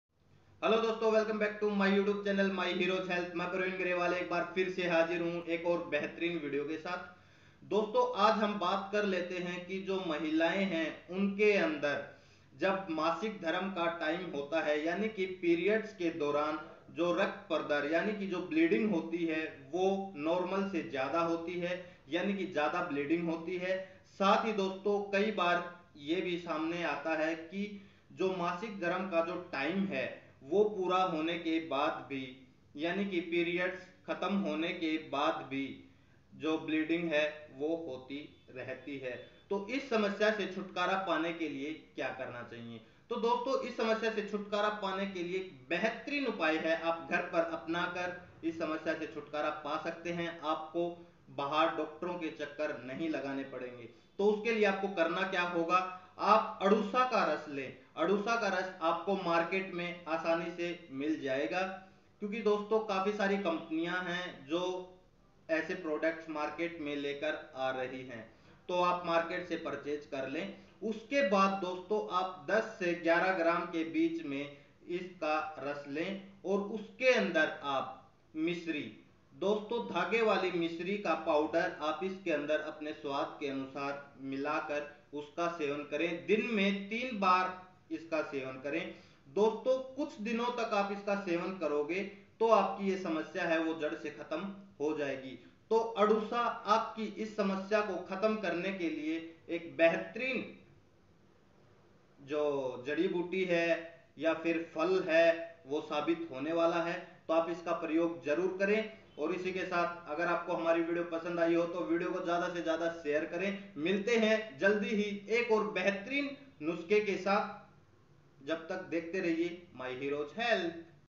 periods me jyada bleeding hove ya periods khatam hone ke baad bhi bleeding ho, deshi gharelu upay
Welcome to 🙏
My Hero's Health
"मेहनत तो करनी ही पड़ेगी"